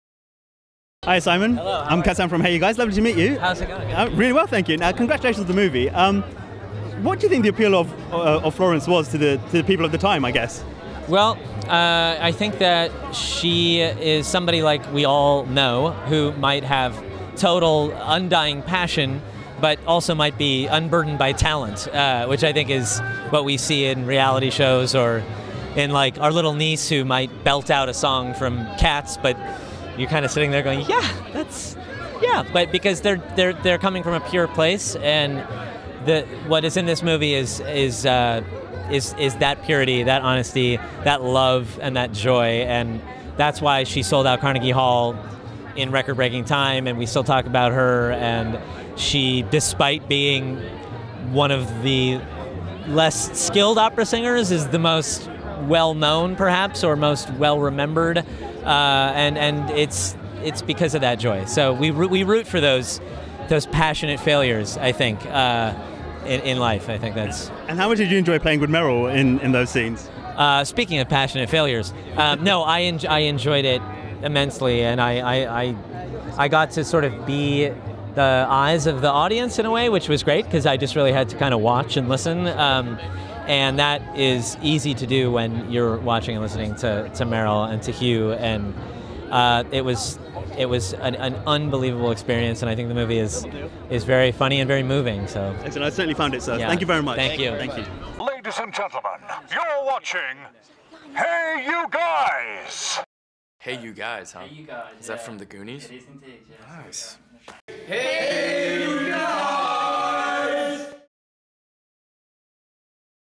Simon Helberg Premiere Interview - Florence Foster Jenkins
Cassam Looch from HeyUGuys interviews The Big Bang Theory star Simon Helberg at the UK premiere of Stephen Frears' Florence Foster Jenkins in London.

Florence Foster Jenkins stars Meryl Streep as the titular songstress with , Rebecca Ferguson, Hugh Grant, Nina Arianda and Simon Helberg.

Plot: The comedy drama tells the inspirational true story of the eponymous New York heiress who obsessively pursued her dream of becoming a great singer. The film celebrates the human spirit, the power of music and the passion of amateurs everywhere

Florence Foster Jenkins is directed by Stephen Frears (Philomena, The Queen) and hits UK cinemas 6th May.